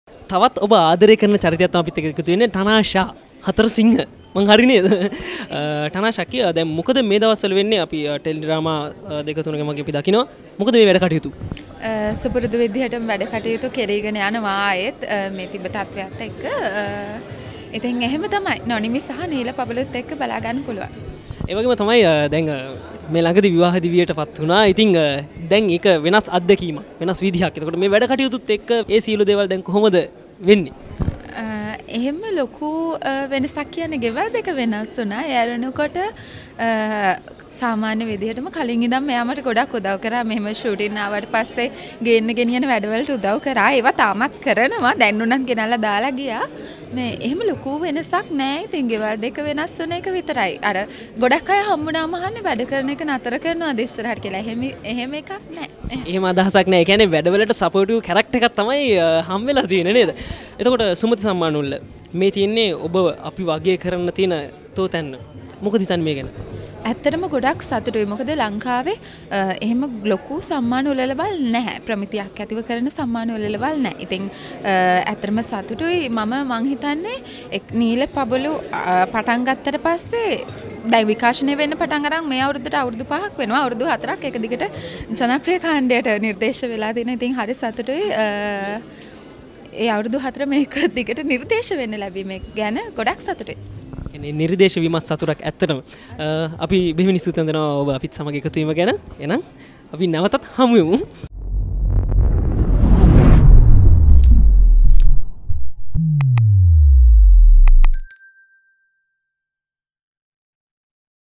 Tanasha Hatharasingha | විවාහ වුනා කියලා රංගනයෙන් ඈත් වීමක් සිද්දවෙන්නේ නෑ | Nonimi |Neela Pabalu
එහෙම ලොකු වෙනසක් නෑ ගෙවල් දෙක මාරු උන එක විතරයි උනේ | Tanasha | Sumathi Awards 2023

2023 සුමති සම්මාන උළෙලේ තවත් විස්තර දැනගන්න...
පහල Link එක Click කරන්න